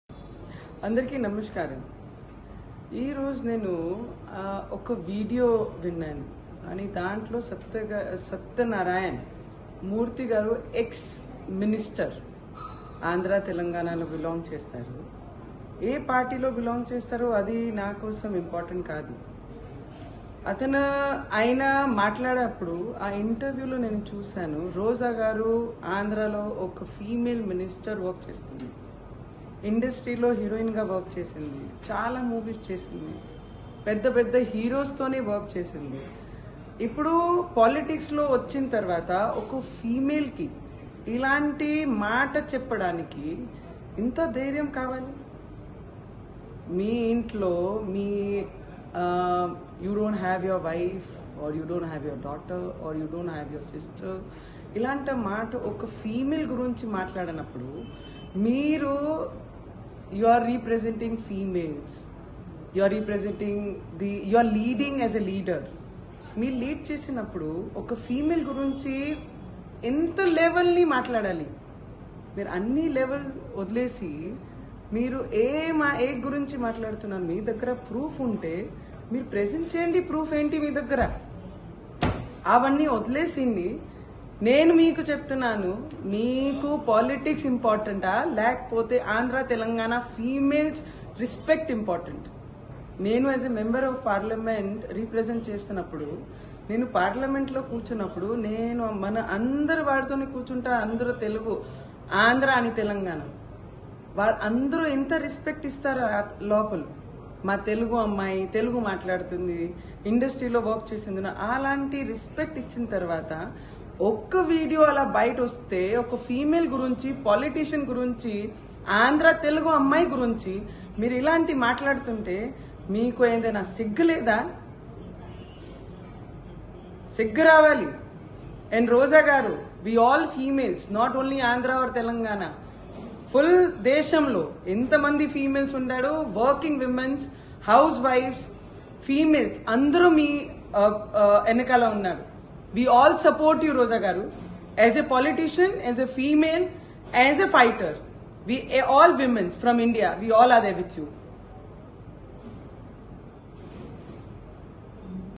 షేమ్ ఆన్ AP పాలిటిక్స్😱😱: MP Navneet Kaur Shocking Comments On Roja and Bandaru Issue😱😱
WATCH : MP Navneet Kaur Shocking Comments On Roja and Bandaru Issue😱😱